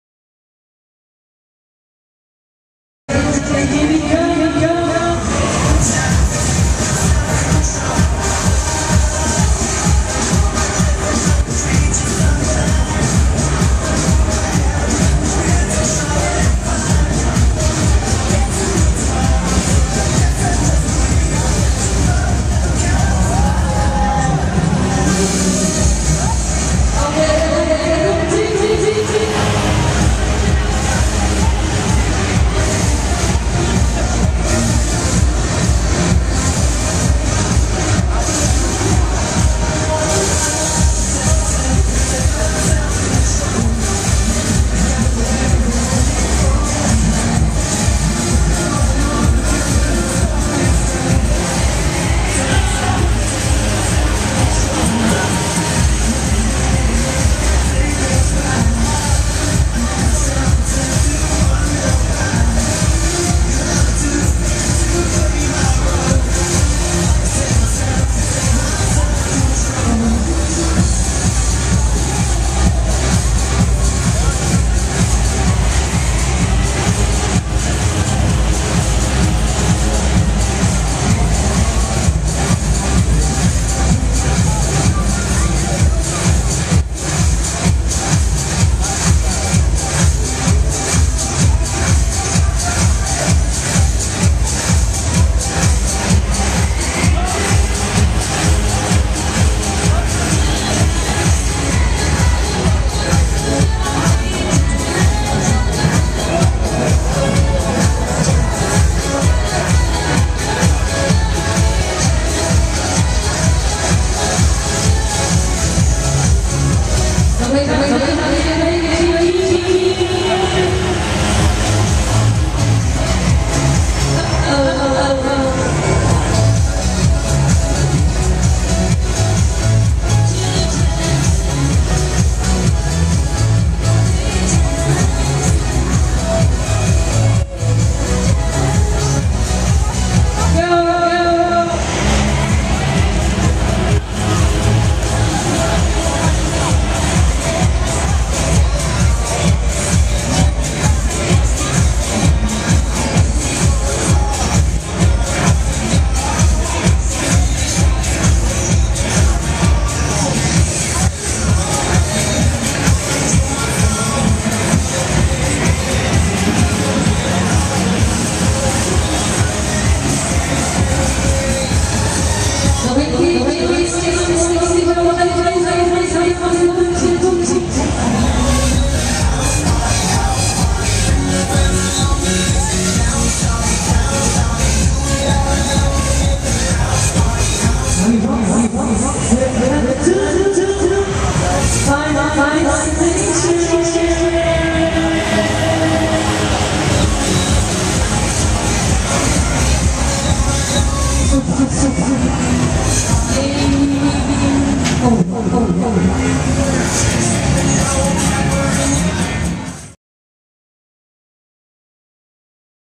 XLR8 Offride Tilburg Kermis 2013
Offride of Otten's KMG XLR8 at Tilburg Kermis 2013, Tilburg Netherlands